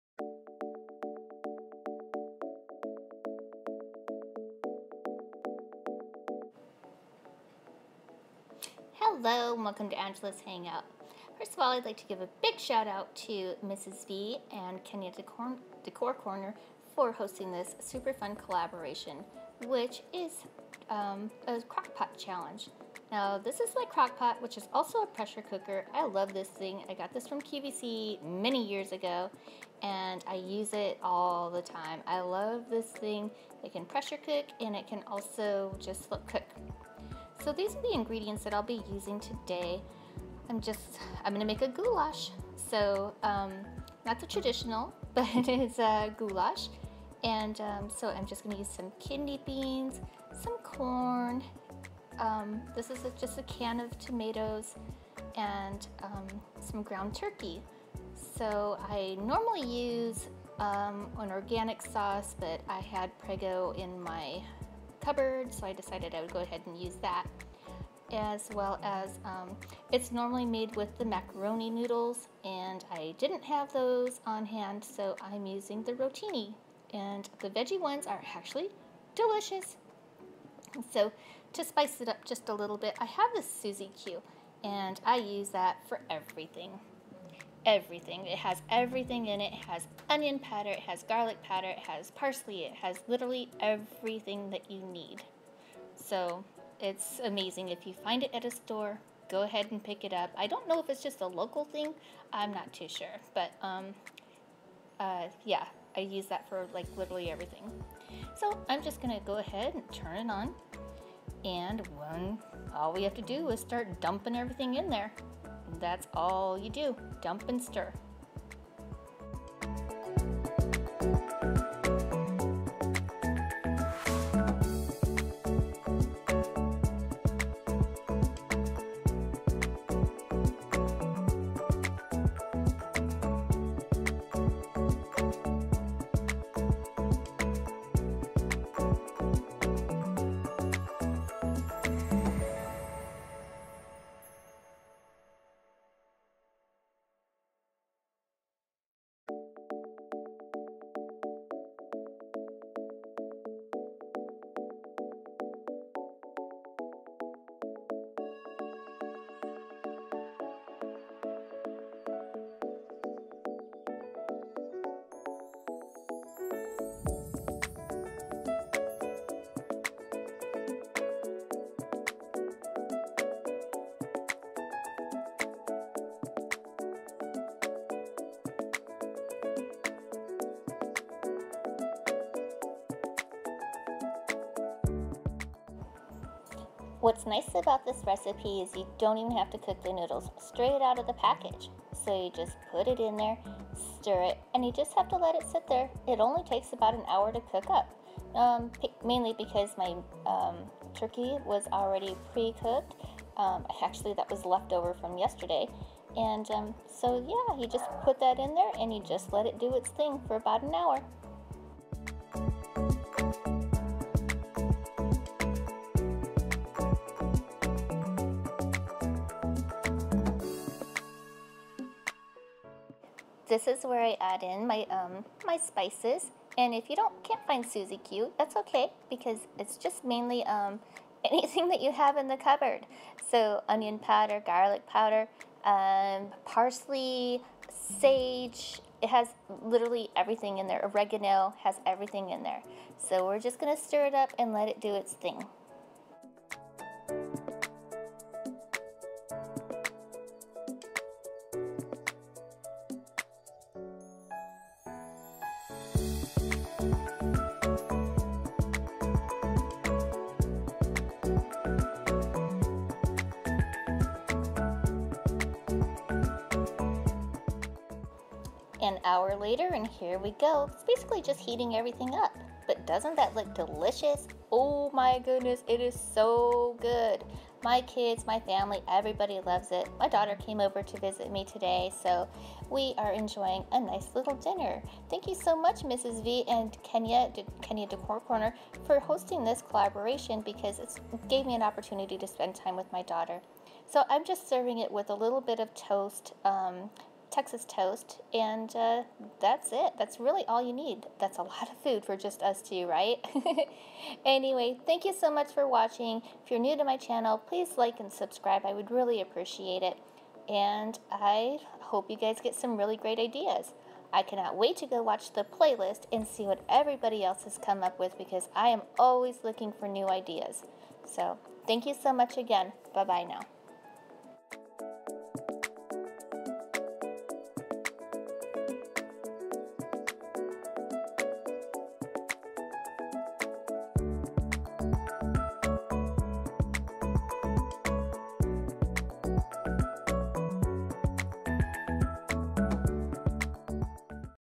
CROCK POT GOULASH | SLOW COOKER CHALLENGE | HOSTED BY: MRS. VEE AND KENYA'S DECOR CORNER
For a quick and easy meal, I always turn to my crock pot.  Today I prepared goulash, not the authentic kind, but one my grandma used to make.  This challenge was being hosted by Mrs. Vee...

As well as Kenya's Decor Corner...

Thank you so much, you lovely ladies for hosting this amazing collaboration.  I can't wait to watch the playlist and get some awesome ideas!!!  Here's the playlist if you'd also like to get some great ideas to put together a lovely crock pot meal...

This recipe is so super easy!  Here is a list of the ingredients I used...

1 lb. Ground turkey (which can be substituted for ground beef or any meat of your choice...or completely omited
1 C  corn
1 C red kidney beans
1 can stewed tomatoes
1 Jar pasta sauce
1 Pkg noodles

Slow cook for 1 hour~ that's it!  So easy!!!  Just dump and wait!  It's a family favorite and my kids just LOVE it!